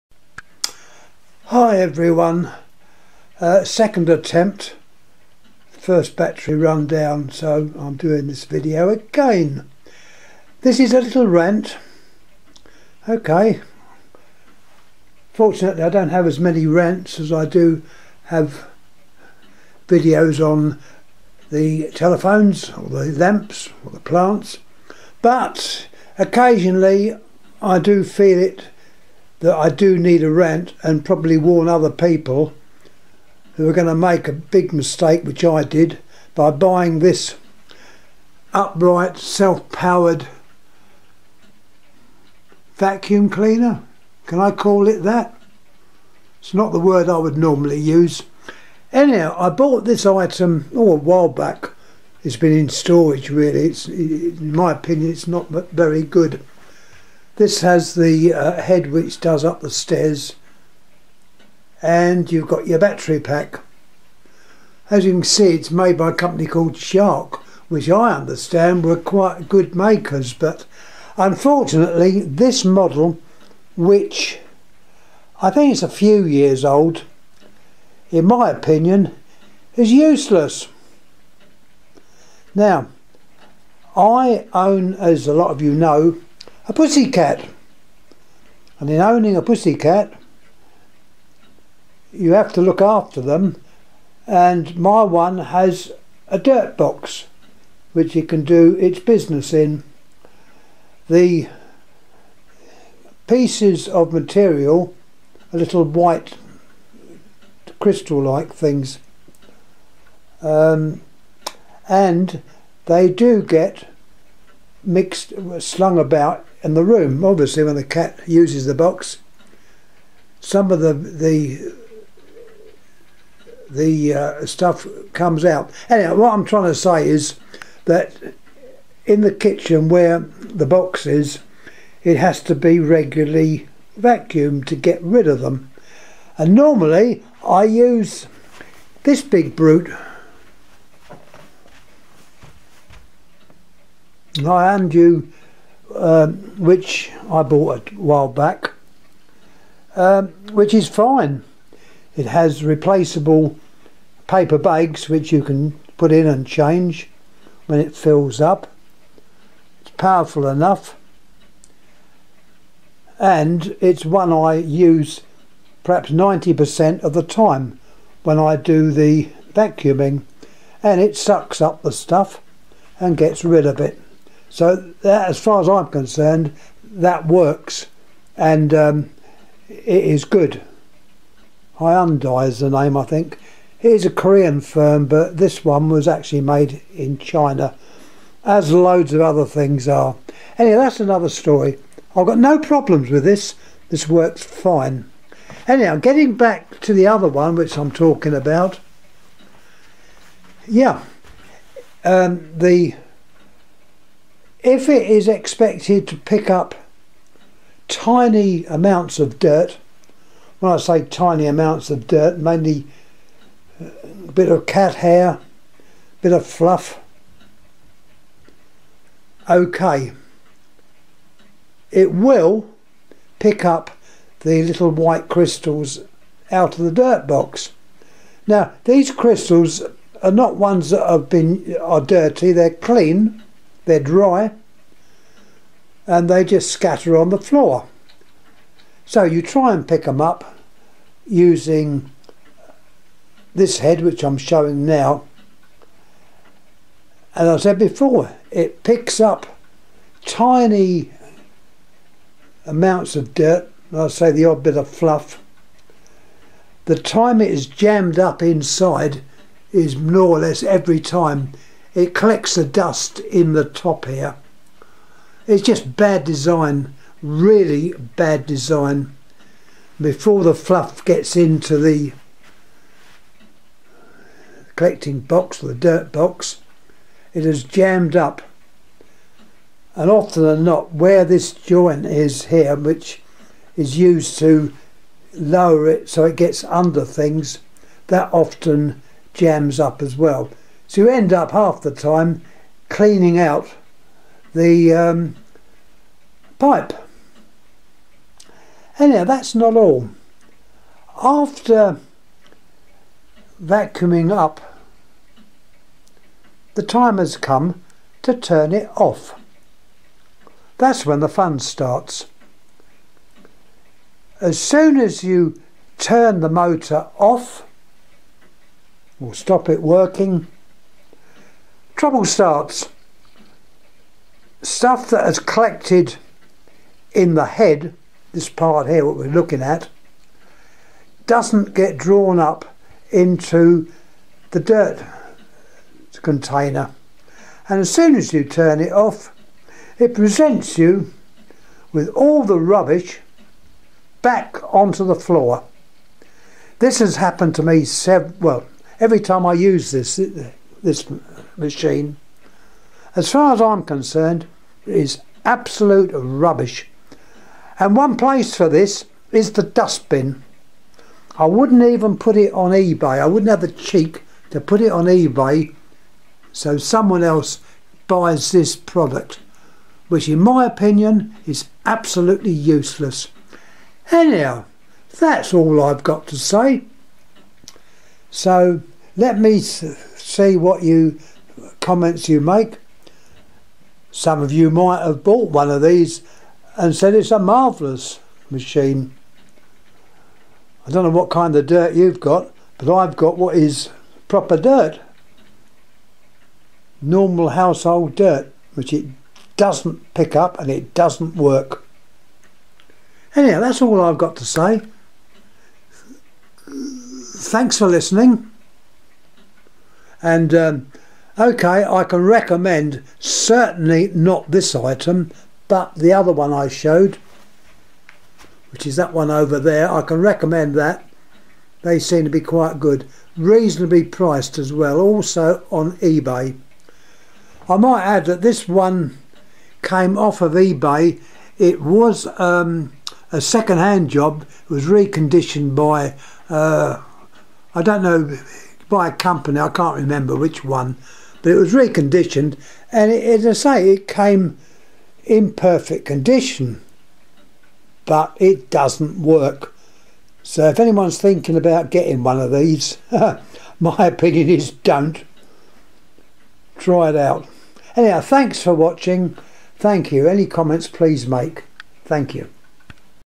Vacuum cleaner rant!
I thought vacuum cleaners were supposed to suck up rubbish not shoot it all out when you turn it off.